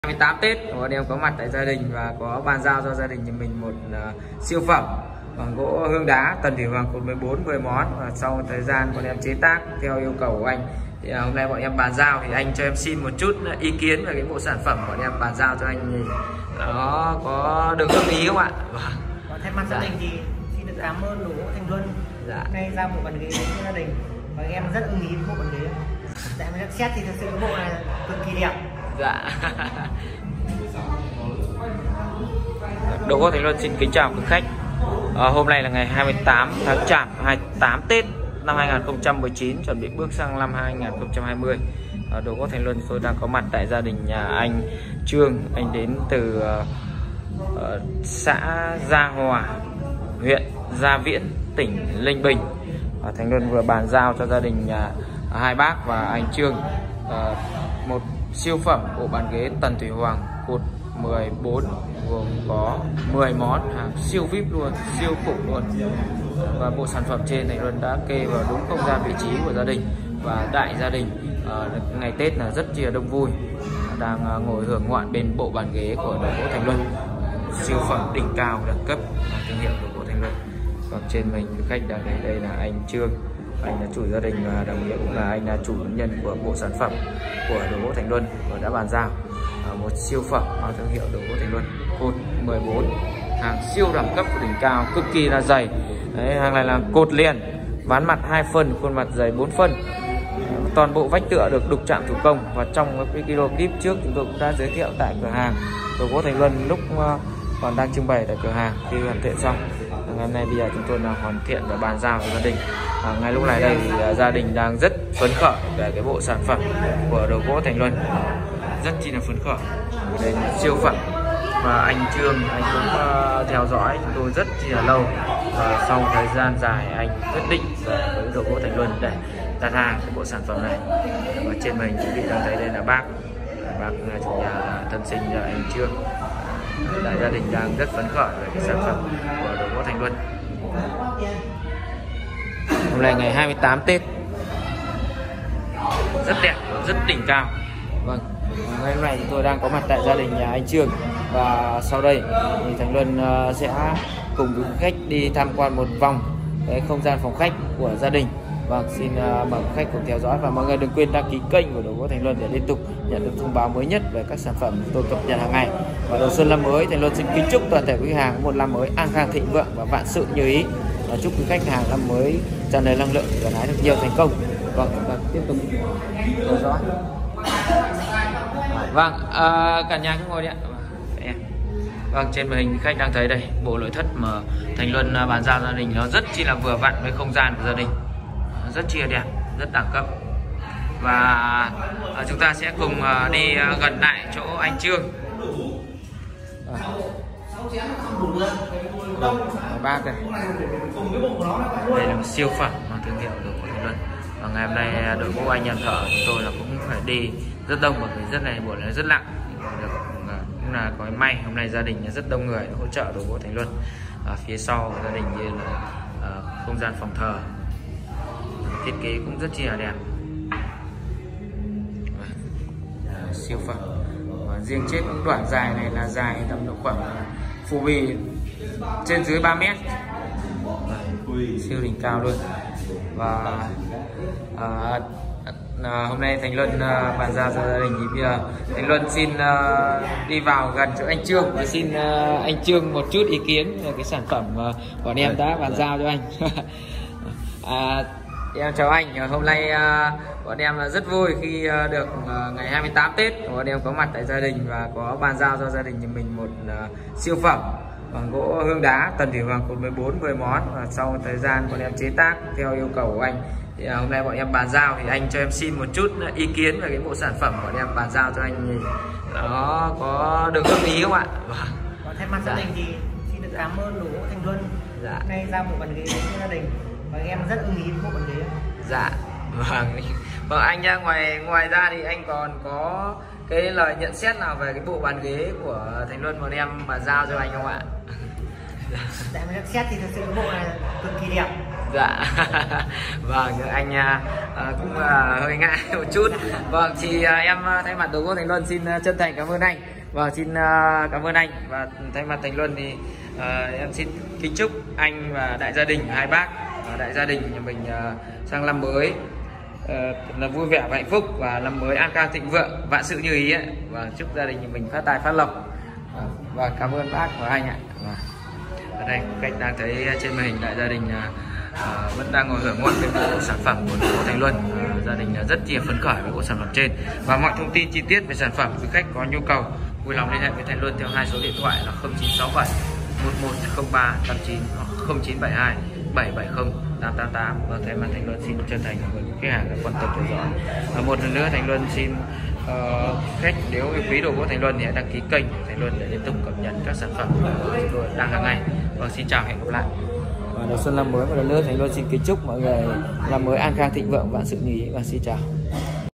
🔴Tết Bàn Giao Bộ Ghế Tần Thuỷ Hoàng Siêu Khủng Cột 14 Gỗ Hương Tại Gia Hoà,Gia Viễn,Ninh Bình
Ngày 28 Tháng Chạp (28 tết 2020) Đồ Gỗ Thành Luân bàn giao và kê xong bộ bàn ghế phòng khách gỗ hương cho gia đình a Cao Xuân Chương tại xã Gia Hoà huyện Gia Viễn tỉnh Ninh Bình.
- Bộ bàn ghế tần thuỷ hoàng gỗ hương đá cột 14 (cột liền) gồm 10 món siêu khủng.
🏢 CS1: làng nghề mộc Thôn Gia Phú xã Tân Dân huyện Phú Xuyên Tp Hà Nội.